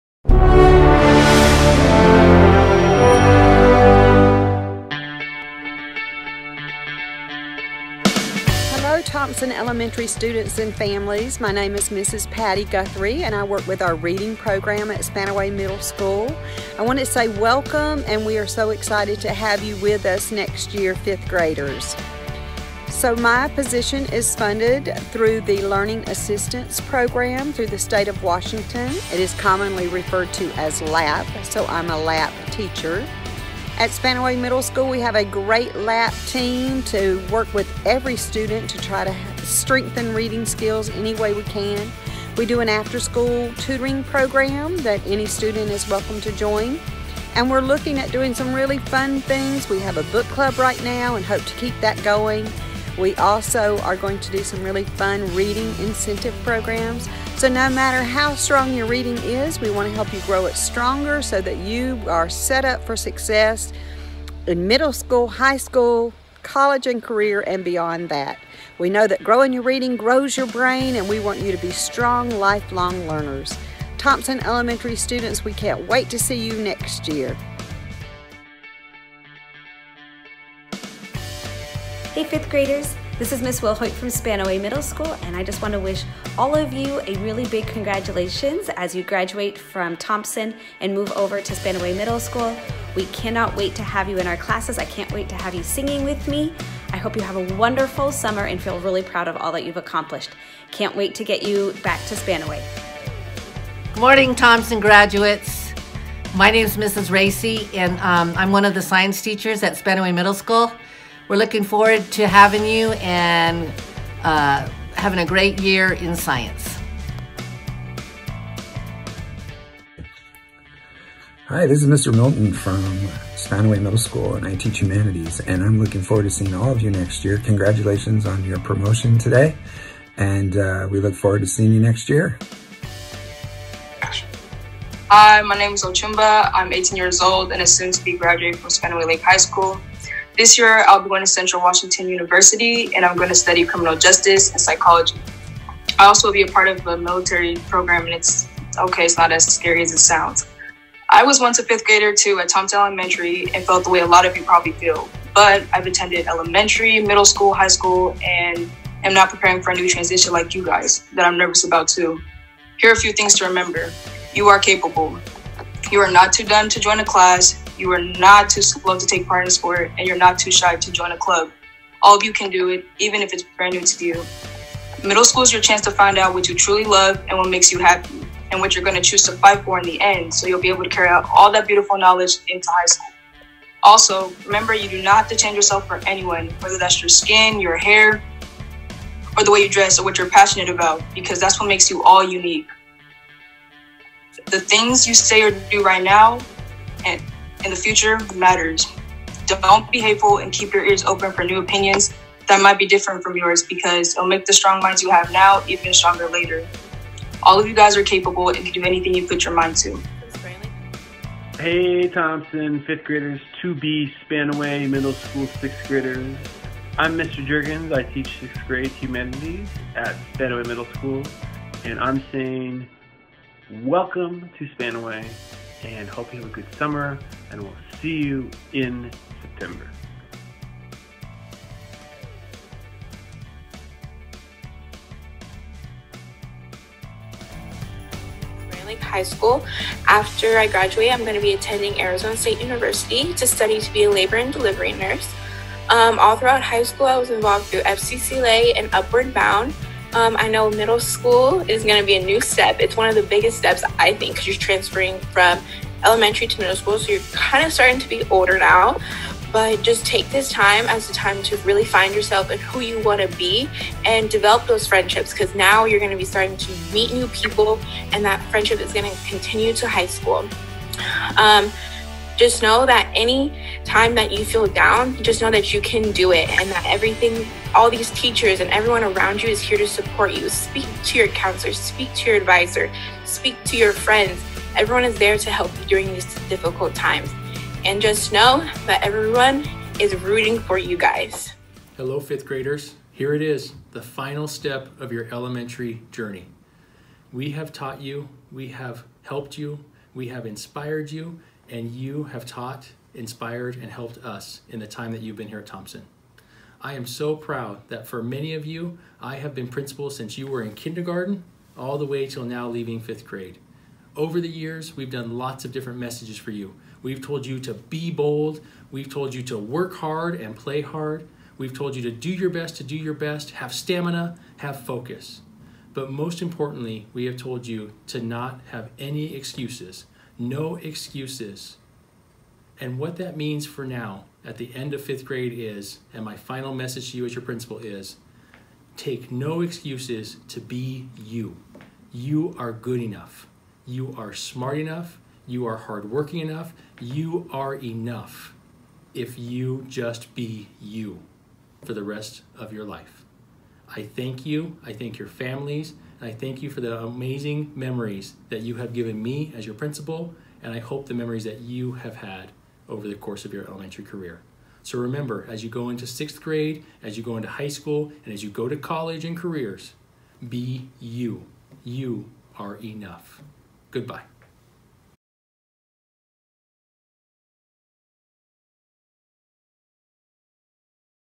5th Grade Promotion!!
Time to celebrate the bright future of our 5th grade graduates!!